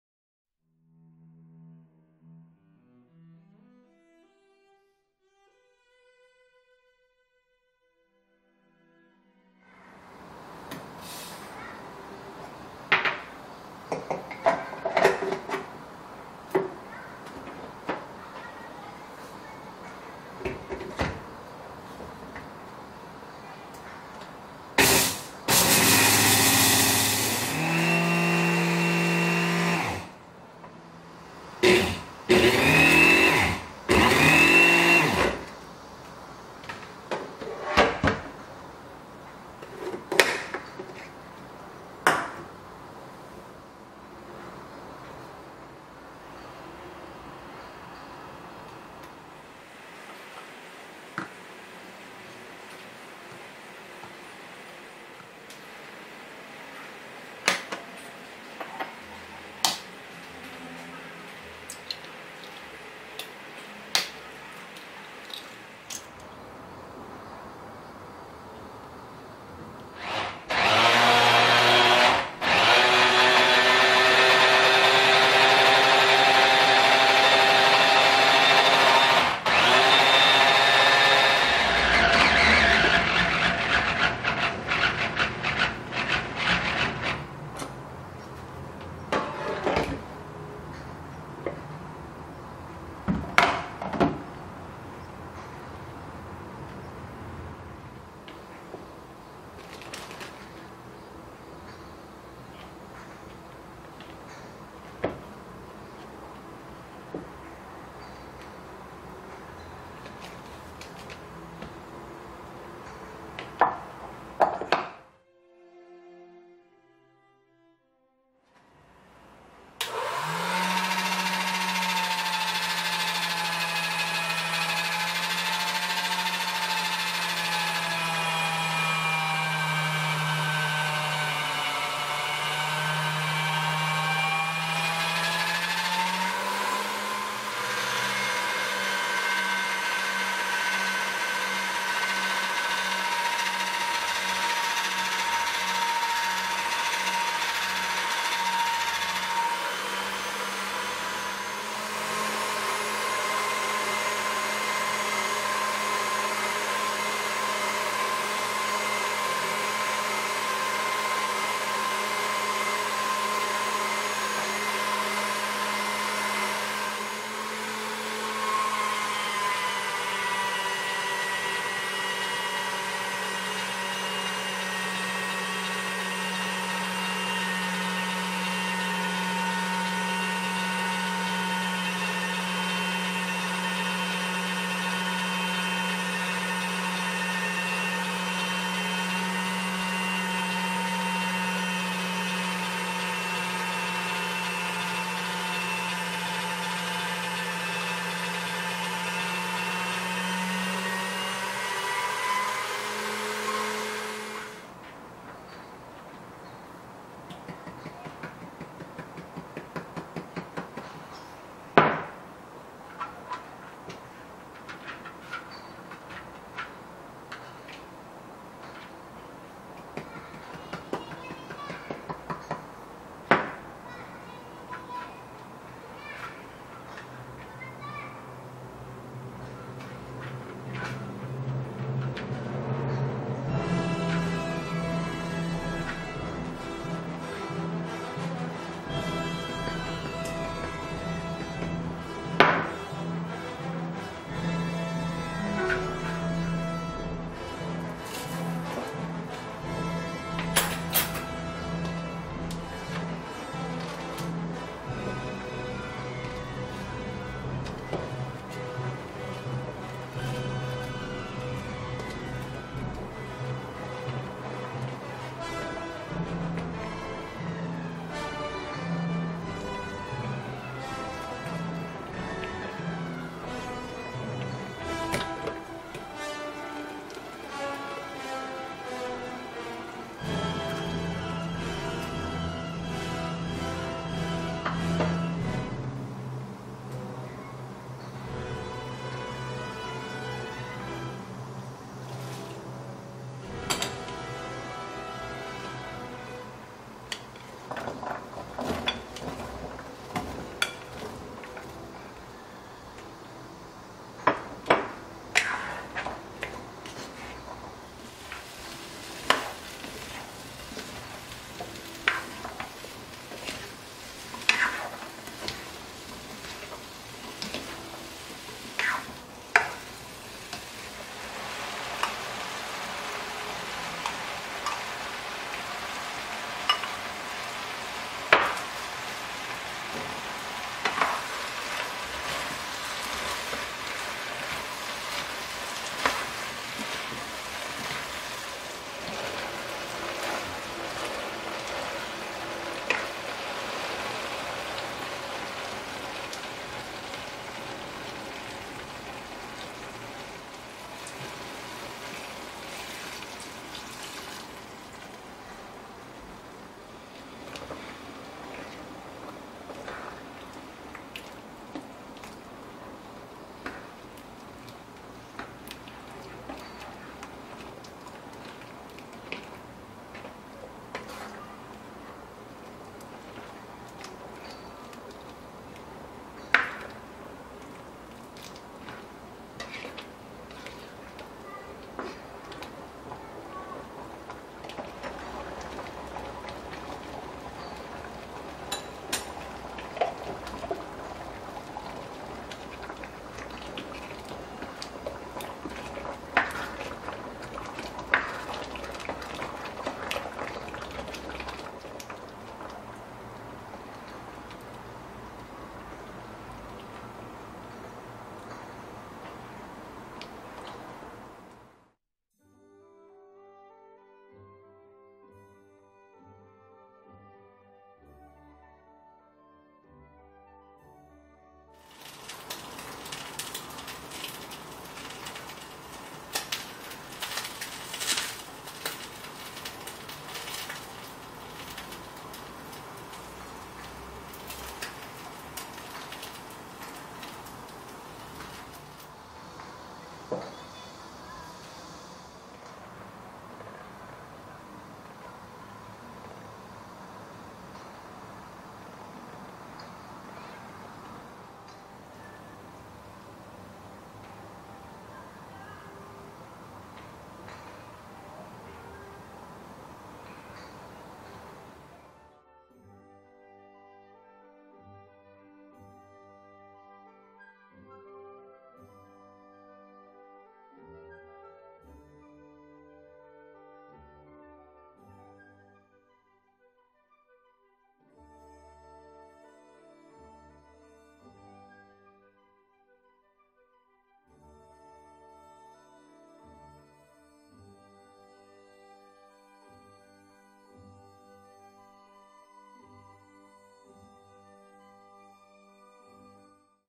카레우동 검정깨국수 만들어 고소한 맛 두배로 만드는법 Curry udon
검정깨 갈아서
생면만들고 강황가루 추가해 찐한맛의 카레우동 만들기!
콩국수였다면
훨씬더 맛있었을것 같다 생각했던 카레우동,

카레는 어떻게 해도 다 맛있다는 결론,